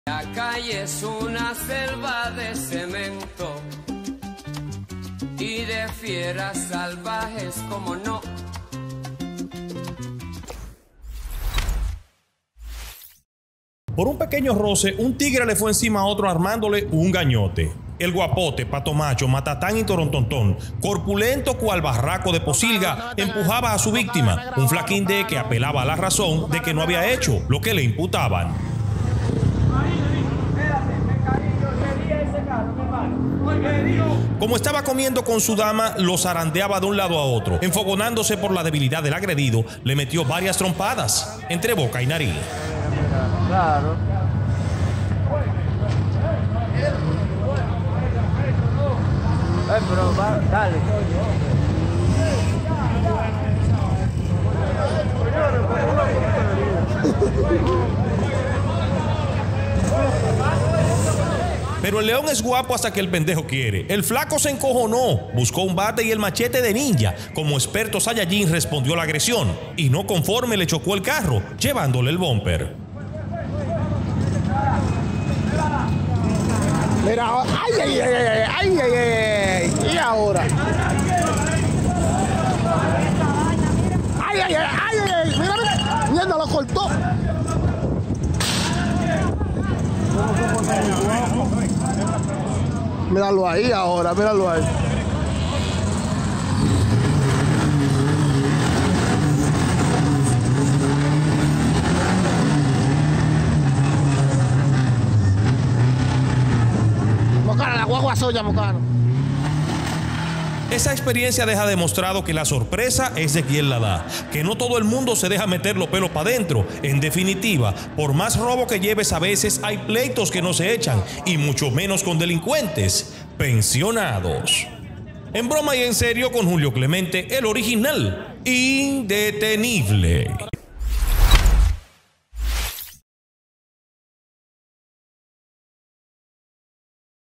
La Sorpresa es de quien la da...
Por un pequeño rose un tiguere le fue encima a otro armándole un gañote, el guapote pato macho matatan y torontonton, corpulento cual barraco de posilga empujaba a su víctima, un flaquinde que apelaba a la razón de que no había hecho lo que le imputaban...